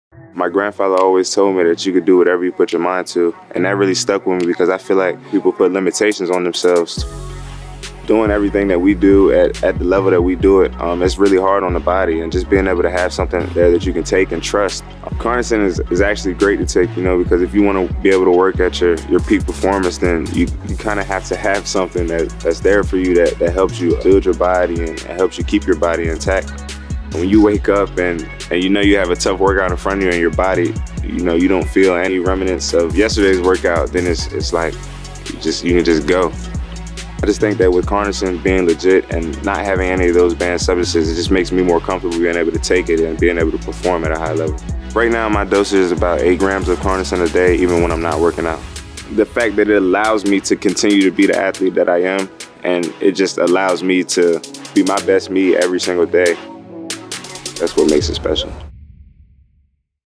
Darnell Savage | No Limits On Performance With CarnoSyn®
Darnell Savage pushes the limits of his performance at TEST Football Academy. Supplementing with CarnoSyn® beta-alanine allows him to be the athlete he strives to be—with increased performance and fast recovery.